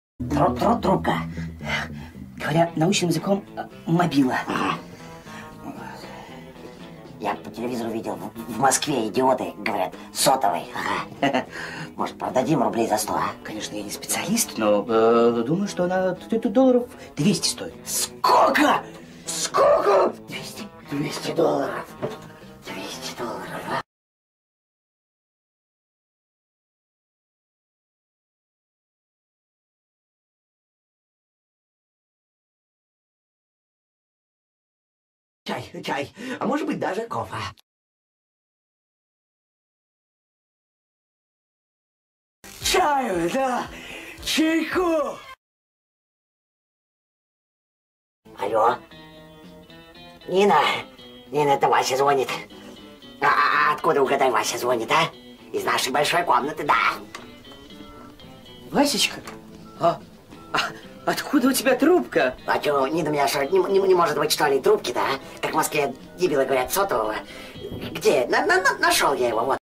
Простой ремонт кнопочного телефона нокиа 1280 (старый надежный телефон nokia 1280)
Плохо нажимались кнопки на телефоне нокиа 1280